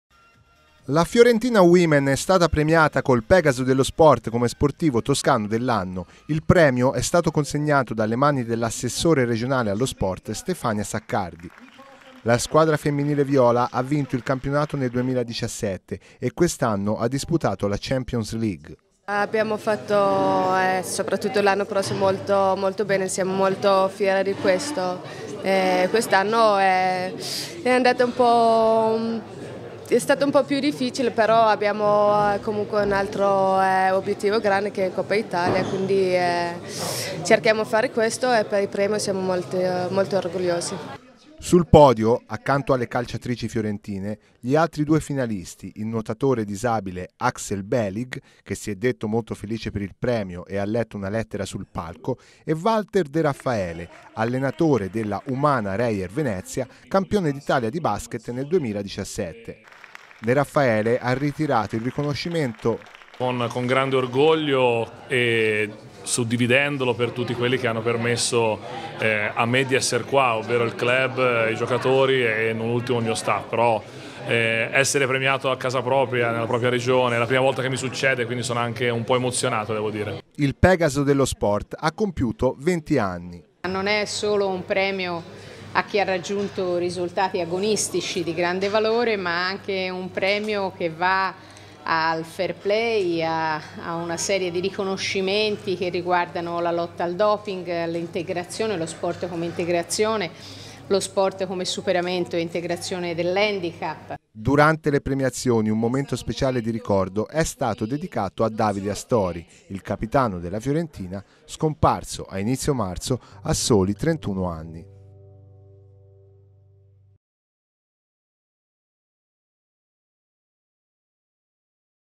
Pegaso dello Sport 2018 alla Fiorentina Women's
L'edizione numero 20 del Pegaso per lo sport va alla la squadra di calcio femminile della Fiorentina Women's, con un ricordo speciale per Davide Astori. La notizia